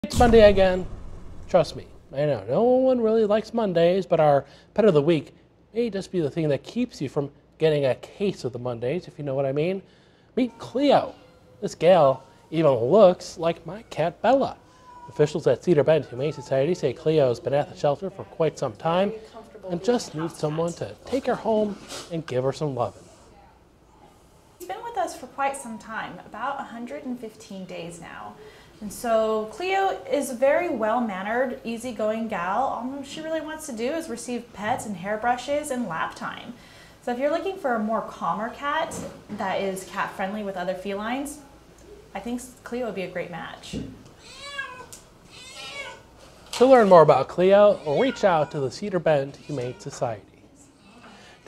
PET OF THE WEEK: Meet Cleo
Officials at Cedar Bend Humane Society say Cleo's been at the shelter for quite some time.